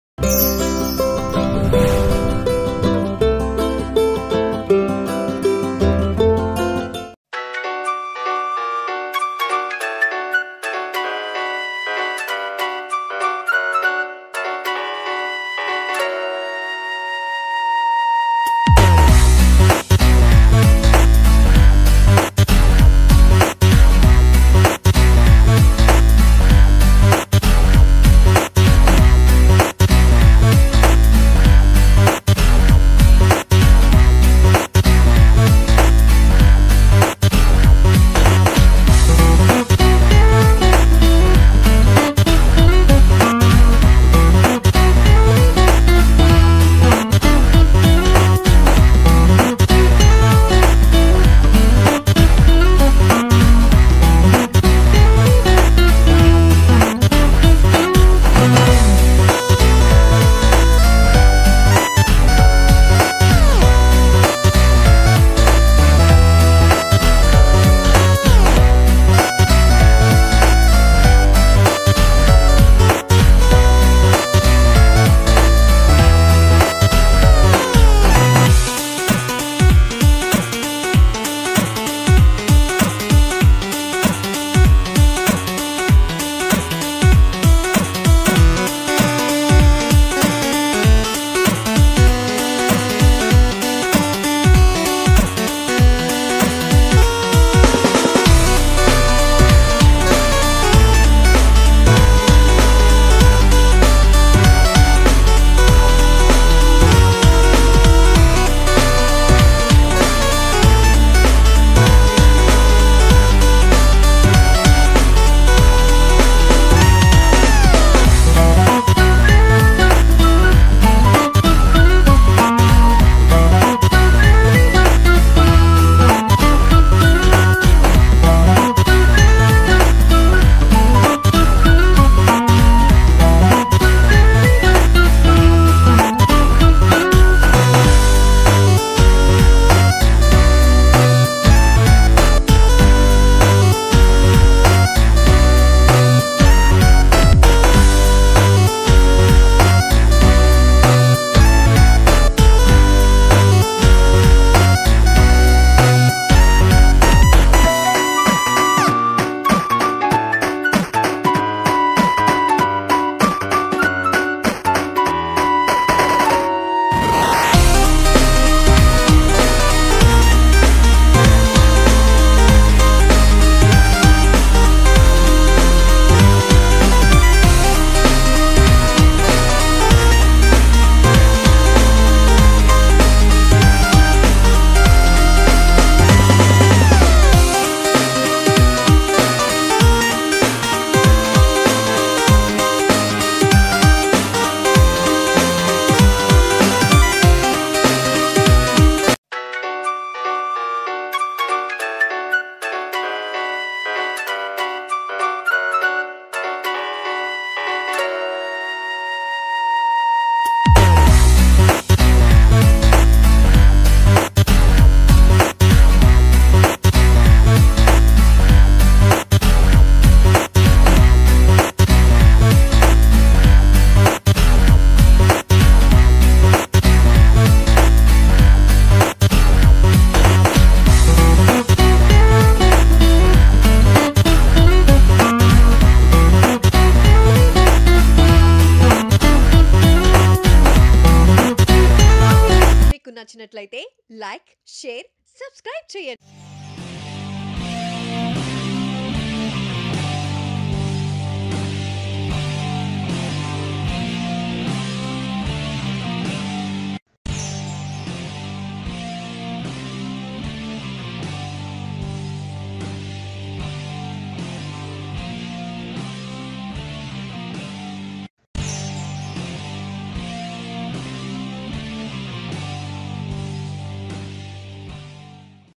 Bigg Boss 3 Contestant Sreemukhi Family Photos | Anchor Sreemukhi With Celebs Photos | News Mantra
Bigg Boss 3 Contestant Sreemukhi Family Photos. Telugu Star Anchor Sreemukhi with Star celebrity photos can watch here.